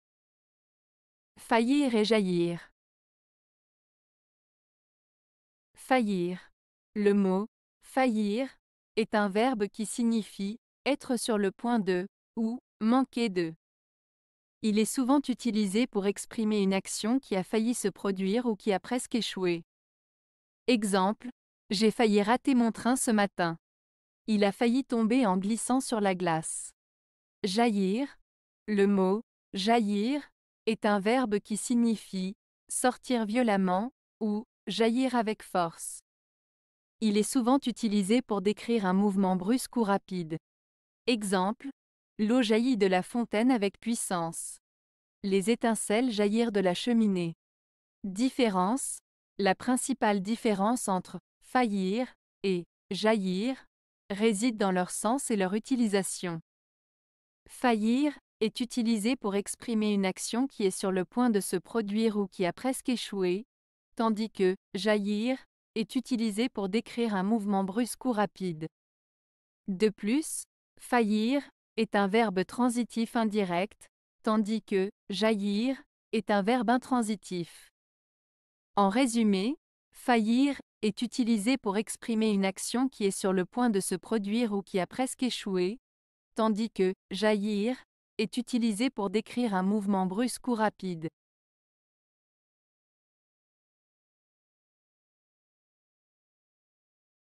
Faillir et Jaillir (Une Lettre)
Faillir:
Le mot "faillir" est un verbe qui signifie "être sur le point de" ou "manquer de". Il est souvent utilisé pour exprimer une action qui a failli se produire ou qui a presque échoué.

Exemples:
- J'ai failli rater mon train ce matin.
- Il a failli tomber en glissant sur la glace.

Jaillir:
Le mot "jaillir" est un verbe qui signifie "sortir violemment" ou "jaillir avec force". Il est souvent utilisé pour décrire un mouvement brusque ou rapide.

Exemples:
- L'eau jaillit de la fontaine avec puissance.
- Les étincelles jaillirent de la cheminée.

Différence:

La principale différence entre "faillir" et "jaillir" réside dans leur sens et leur utilisation. "Faillir" est utilisé pour exprimer une action qui est sur le point de se produire ou qui a presque échoué, tandis que "jaillir" est utilisé pour décrire un mouvement brusque ou rapide. De plus, "faillir" est un verbe transitif indirect, tandis que "jaillir" est un verbe intransitif.

En résumé, "faillir" est utilisé pour exprimer une action qui est sur le point de se produire ou qui a presque échoué, tandis que "jaillir" est utilisé pour décrire un mouvement brusque ou rapide.